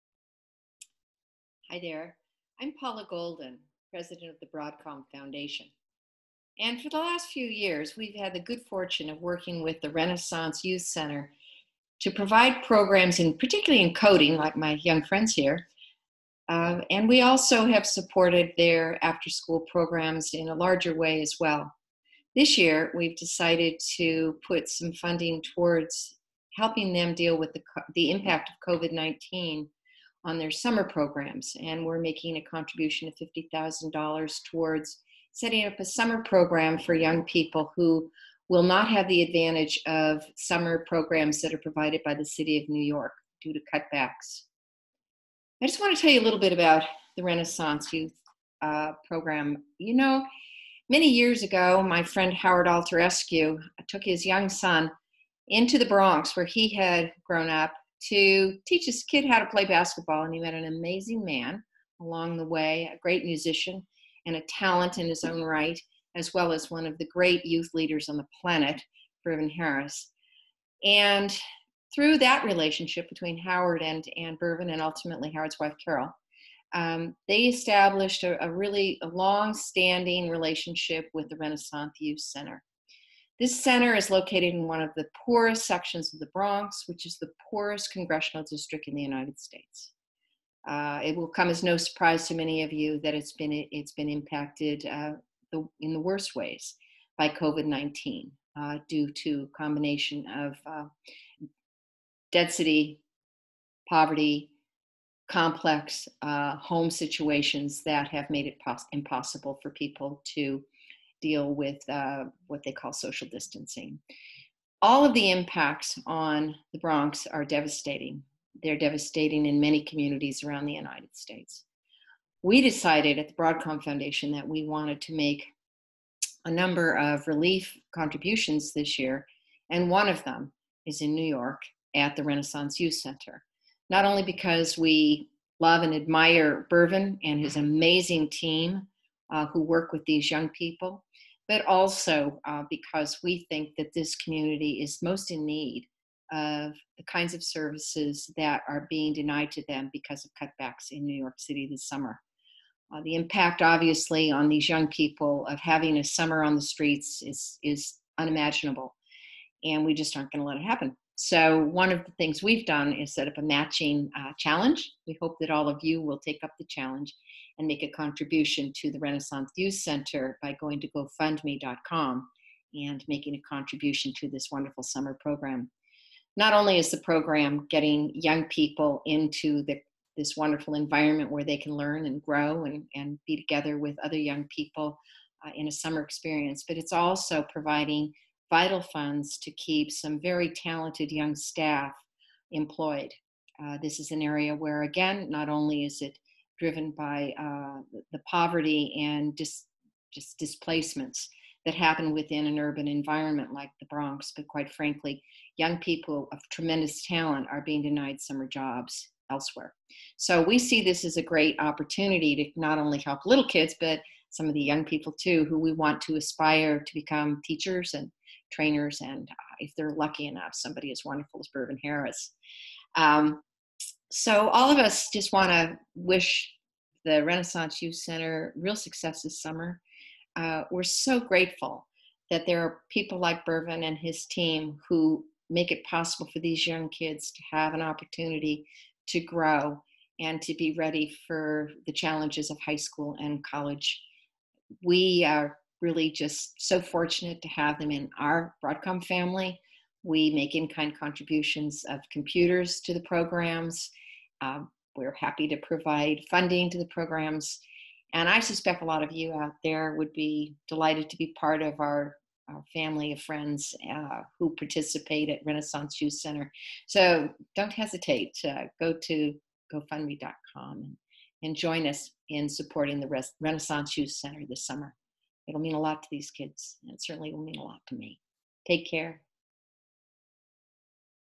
Learn Why Broadcom Foundation Supports Bronx Summer Program 2020
Hear from Paula Golden, President of Broadcom Foundation on our decision to match every donation (up to $50K) to provide a summer program in the Bronx for 100 kids.

New York City has cancelled funding for most summer programs this year, so please help Renaissance Youth Center (RYC) give Bronx youth a safe and engaging place to be this summer.